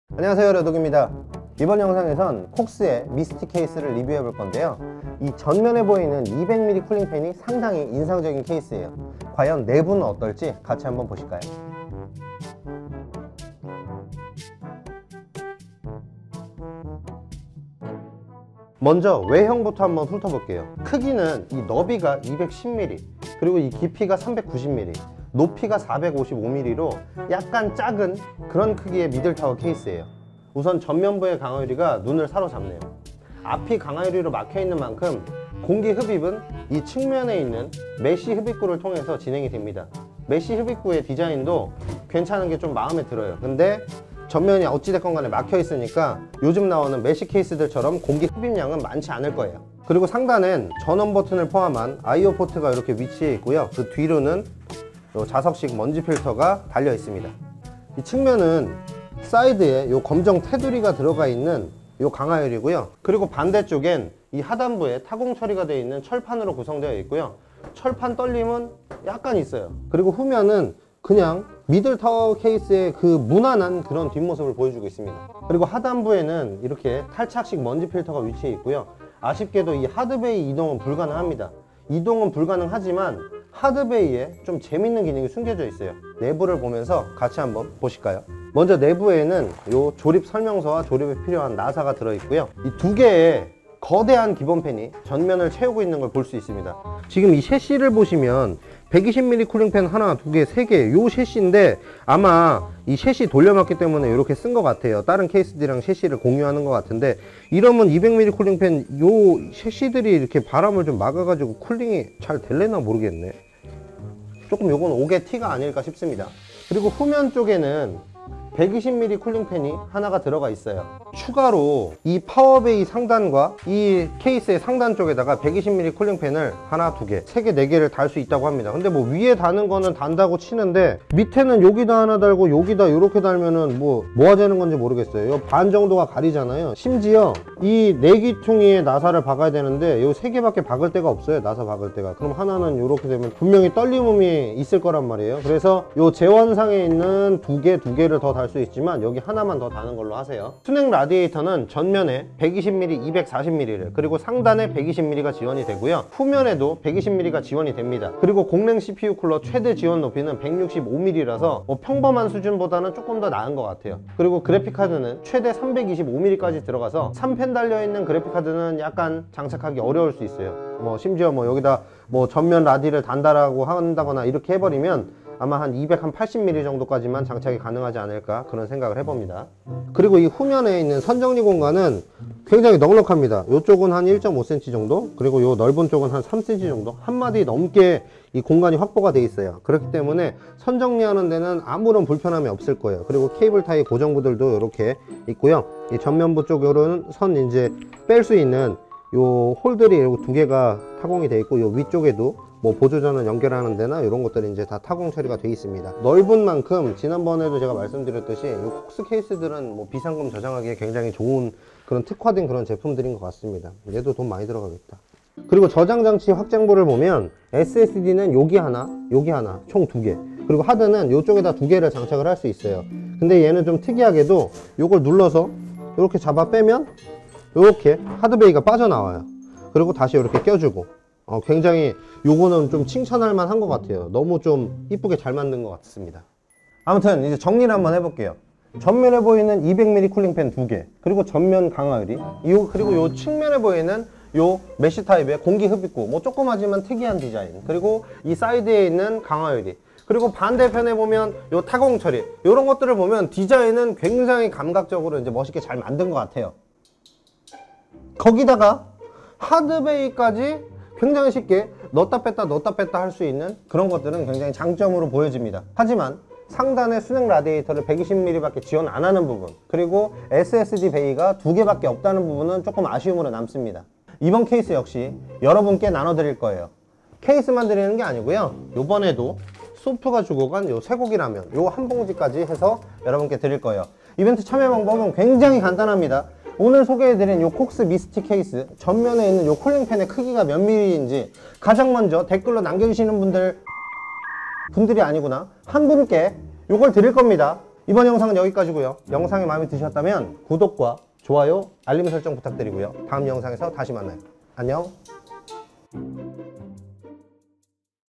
COX A6 미스틱 케이스 리뷰! - (200mm 기본팬이 두개??)
이번영상에선 콕스의 A6 미스틱 케이스를 리뷰해볼거에요
마음에 들면서도 약간 애매한 케이스인데요
과연 어떨지 한번 같이 보실까요?

＊댓글 수정하시는 분들은 당첨에서 제외되니 절대로 수정하시면 안되요!!